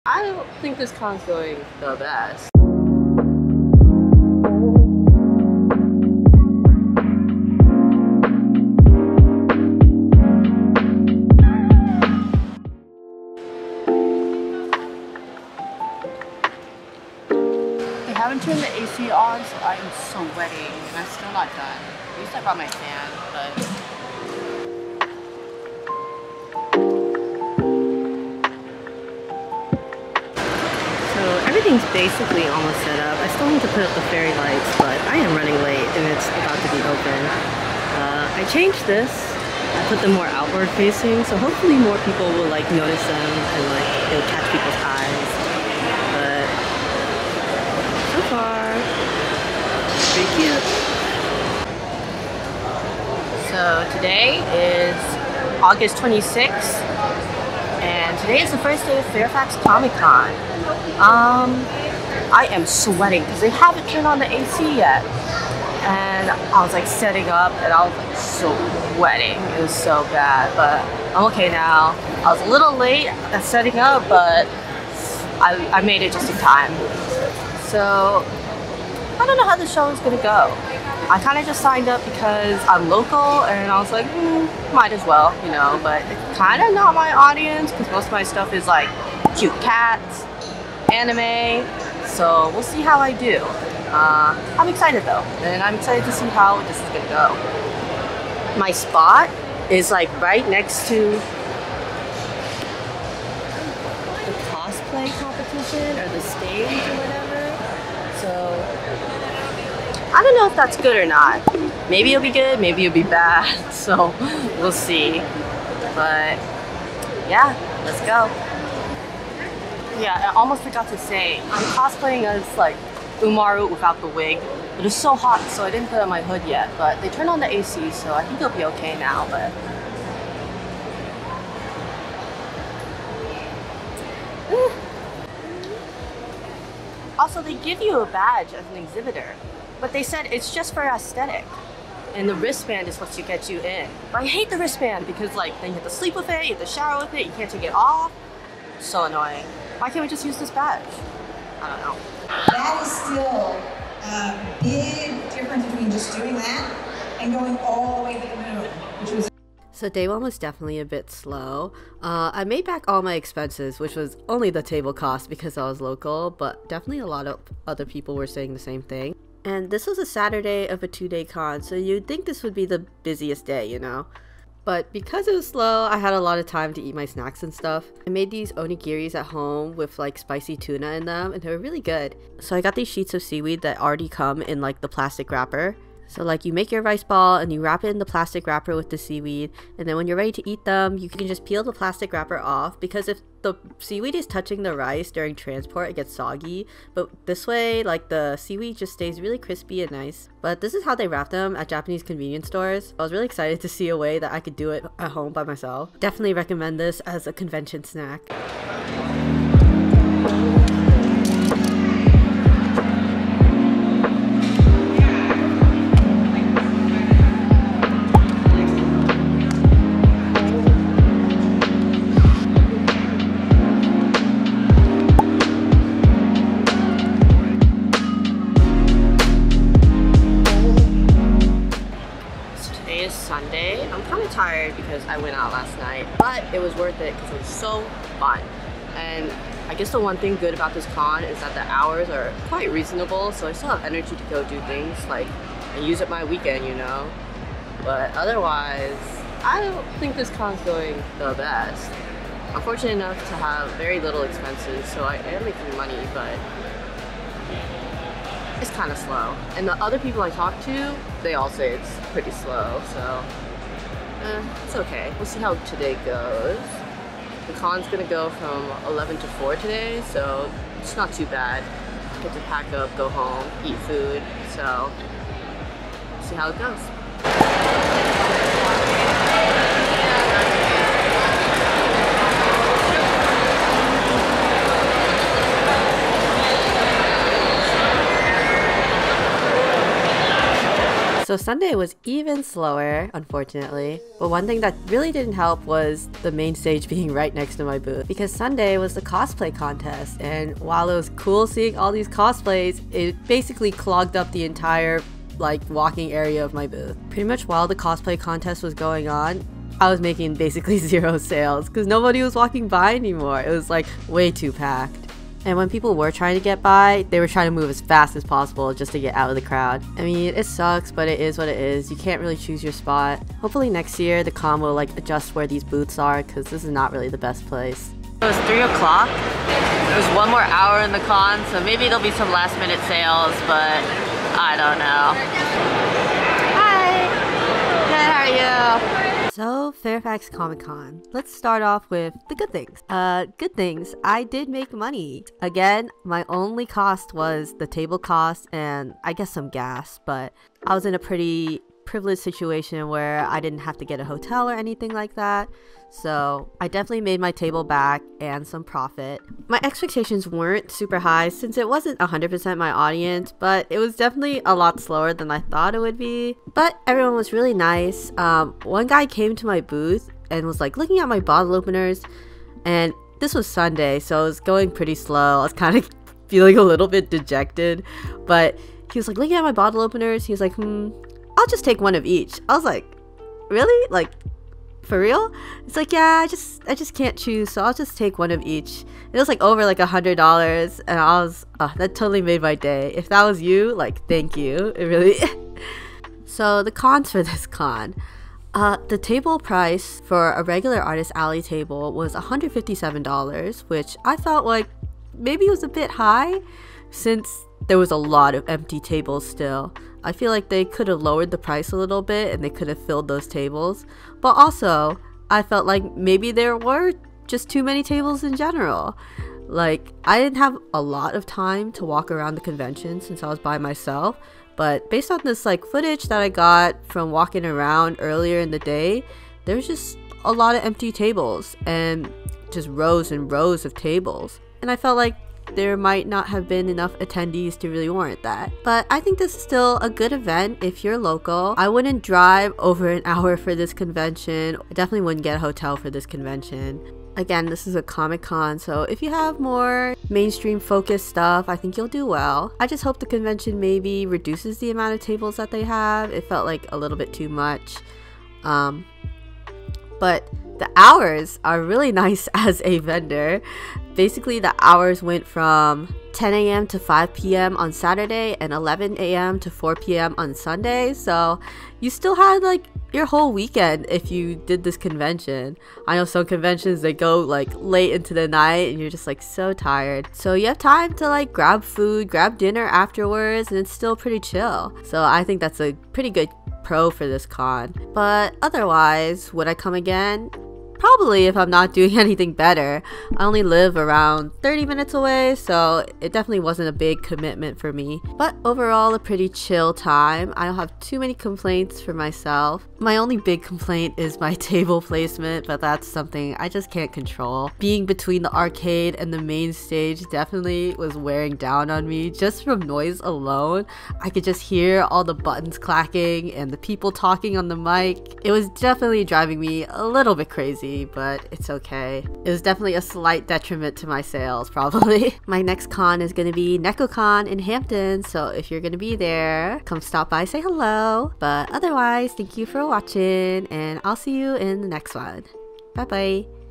Are Local Cons Worth? || Fairfax Comic Con 2023 Artist Alley Vlog
Fairfax Comic Con was chill time but I just want to do more anime conventions :')

Time Stamps:
0:00 Intro
0:12 Day 1
3:28 Day 2
5:39 Debrief and thoughts on the con

Sticker display
Plastic Grid cubes
Metal wire gird cubes
Dish rack print display
Keychain Adhesive Hooks
Foldable table
Tall camping chair